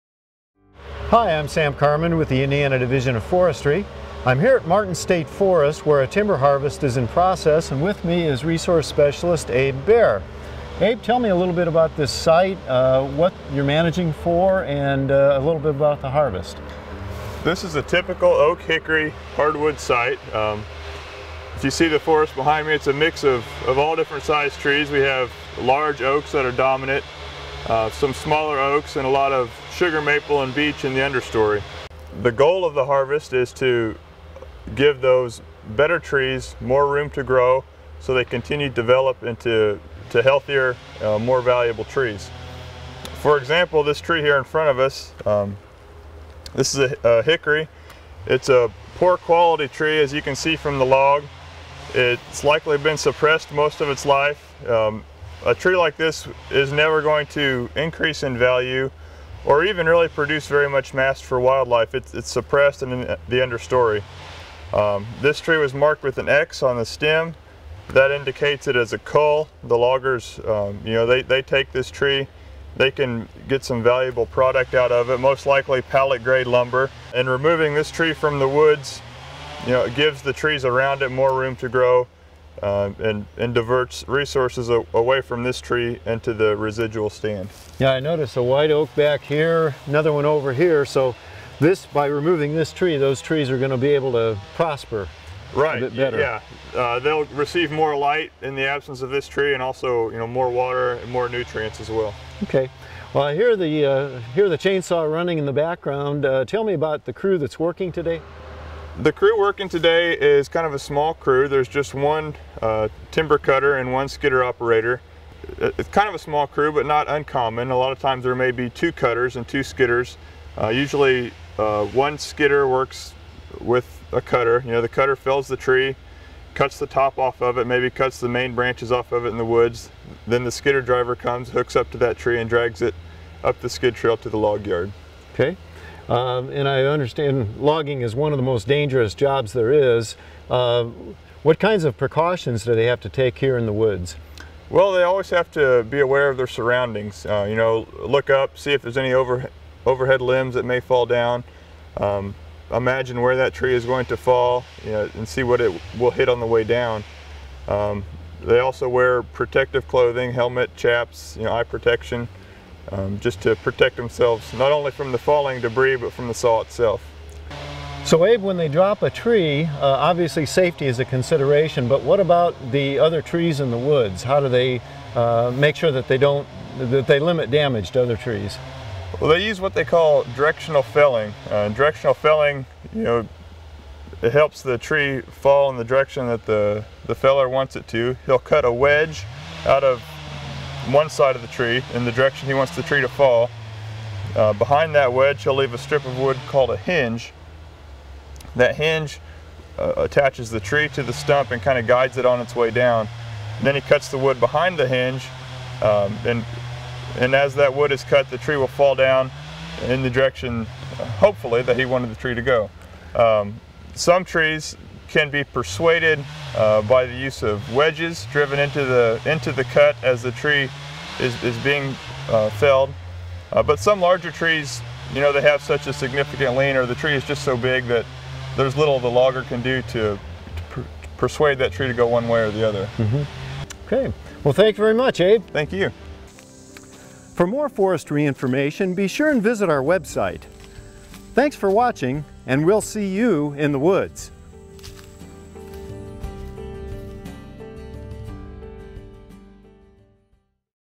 Timber Harvest at Martin State Forest | Indiana DNR
Sam Carman meets up with resource specalist Abe Bear to learn about managing a timber harvest at Martin State Forest.